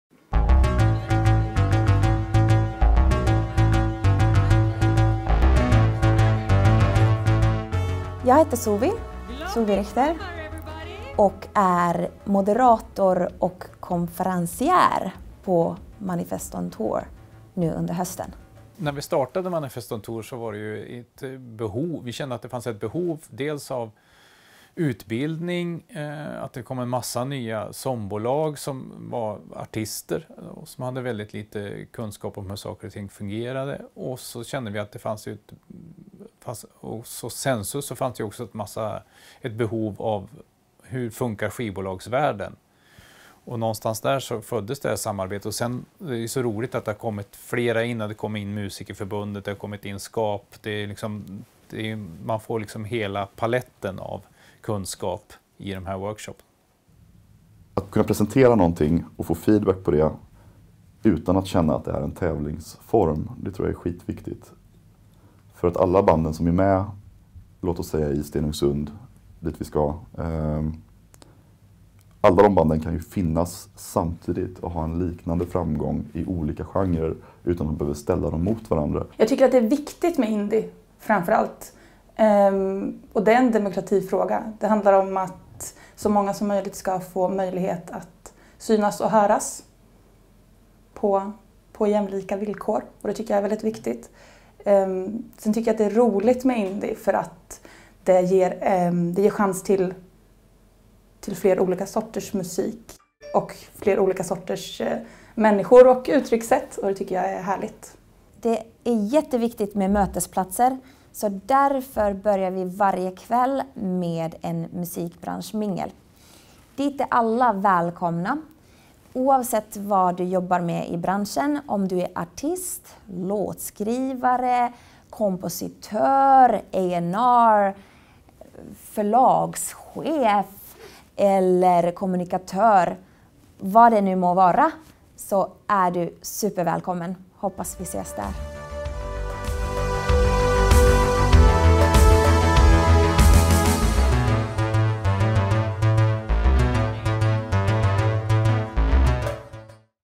Manifest on Tour - 2018
Manifest on Tour är en turnerande musikbranschworkshop som under hösten kommer besöka sju olika städer utspridda i Sverige från norr till söder.

Syftet med turnén är att lokala band och artister runt om i landet får tillfälle att spela live och träffa musikbranschen och sin publik för att få mer erfarenhet att ta med sig vidare i musiklivet. Turnéns syfte är också att ge dagens musikbransch en insyn och möjligheten att ta del av nya band och artister som kan leda till framtida samarbeten.

12/10, Luleå
18/10 Malmö
19/10 Stenungsund
20/10 Västervik
8/11 Stockholm
9/11 Östersund
10/11 Gävle

Manifest on Tour arrangeras av SOM och Sensus Musik i samarbete med Svensk Live, SKAP och Musikerförbundet.